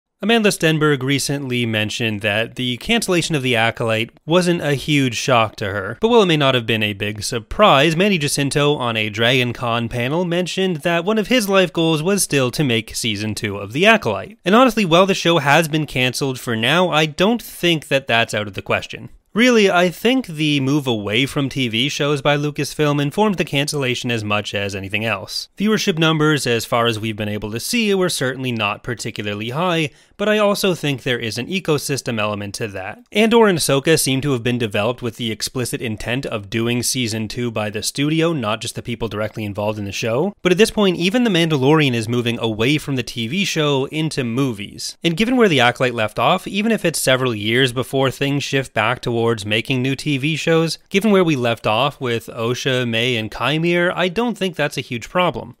There's hope for the Acolyte
The Acolyte is cancelled but Manny Jacinto still has hope

Star Wars Clips, Star Wars Lore, Acolyte, High Republic, Qimiur, Sith, The Acolyte